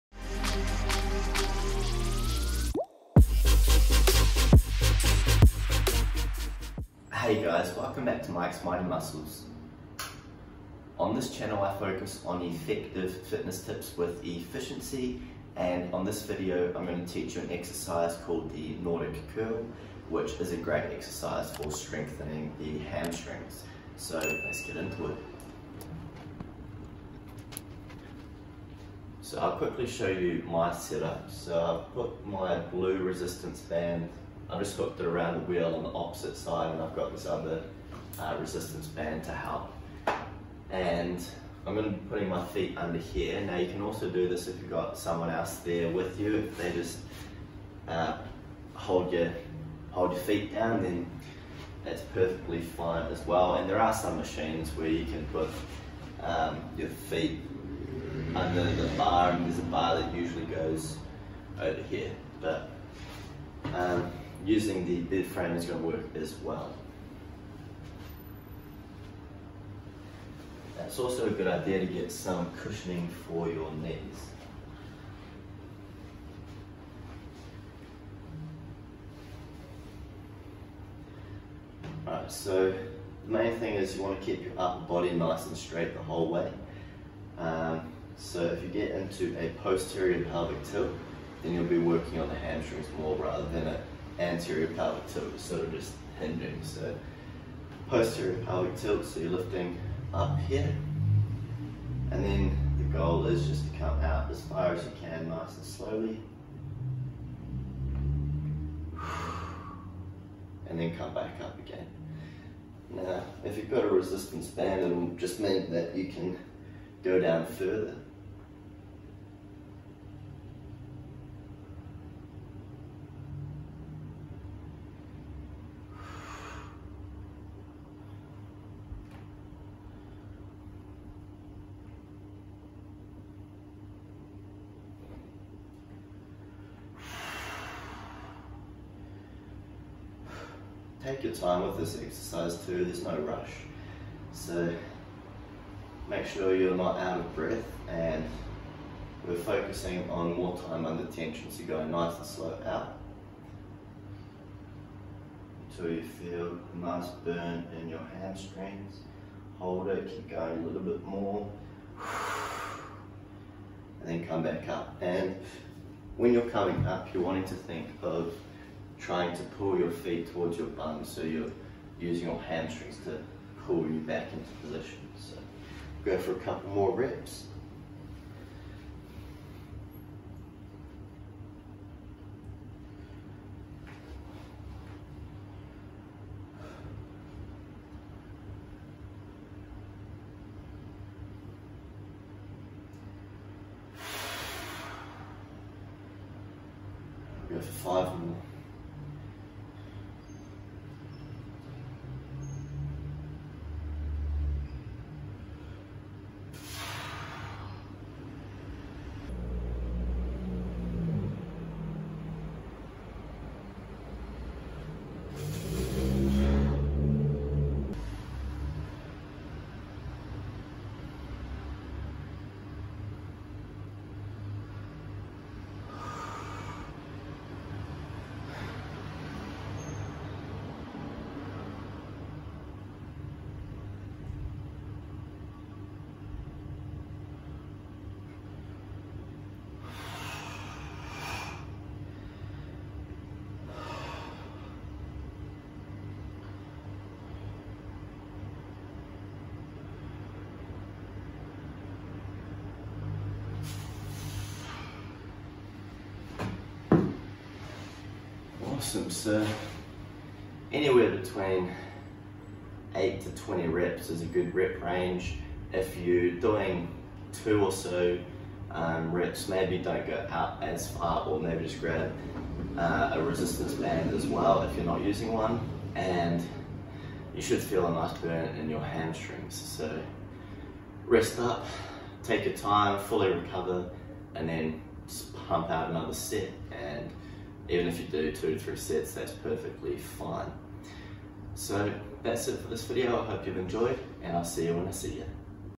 How To Do Nordic Curls (Hamstring Strengthening)
In this video I share an easy way to get a hamstring workout at home.